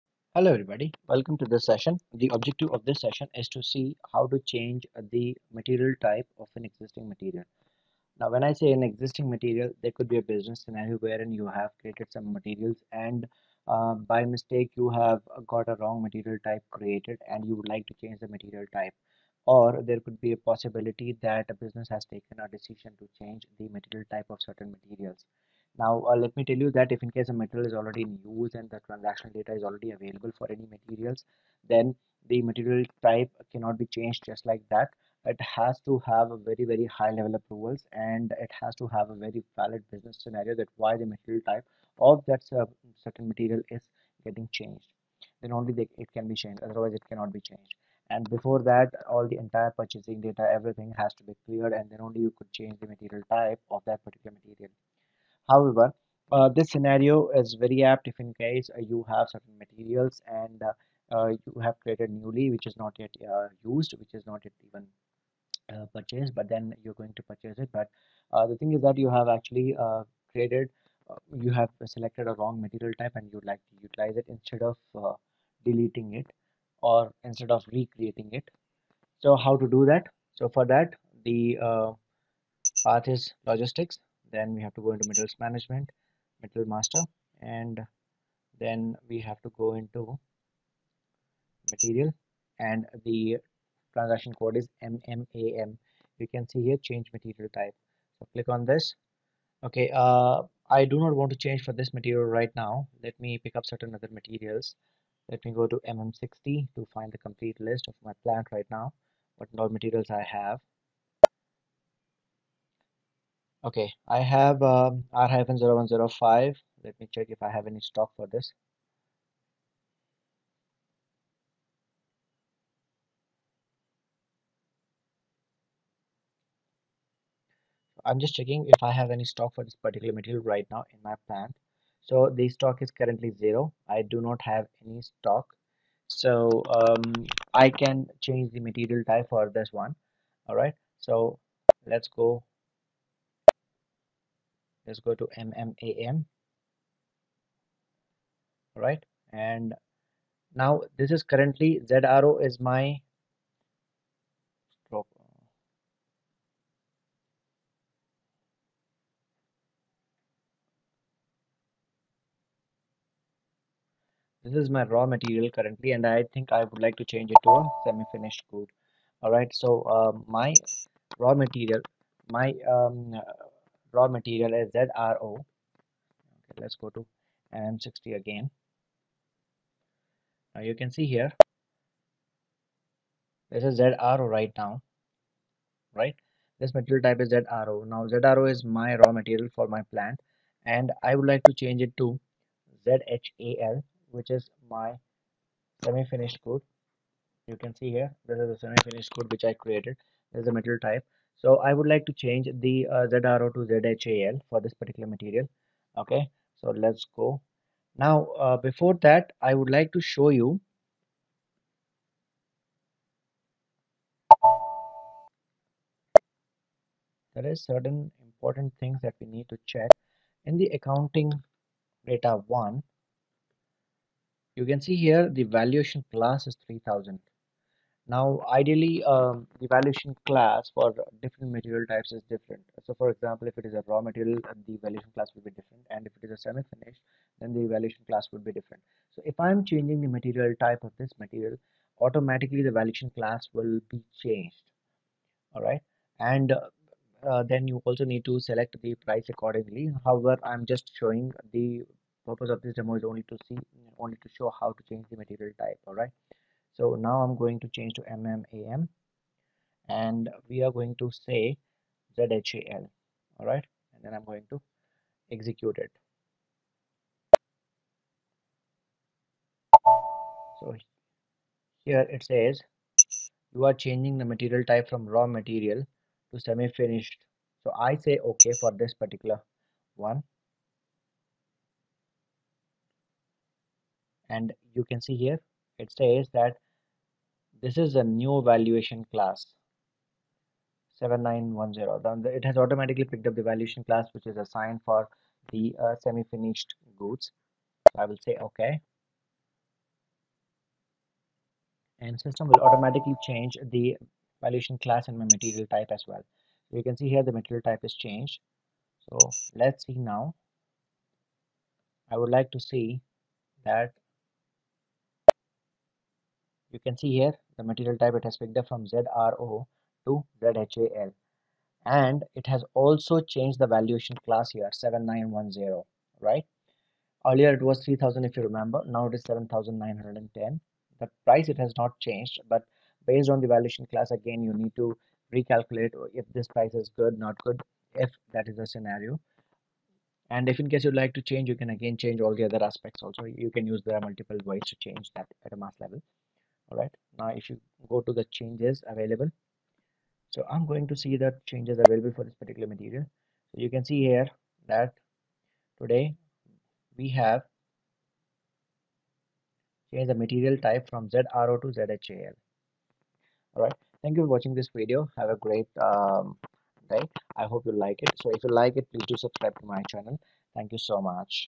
How to change the material type of a material in SAP - SAP MM Videos
This video talks on how to change the material type of an existing material. If new materials are created with wrong material types by mistake, this tool is really helpful to change it.This could be useful in any other scenarios as well.

One important point is that you can not change the material type of an existing material just like that. It needs high level business approvals.

Important TCodes:
Change Material Types: MMAM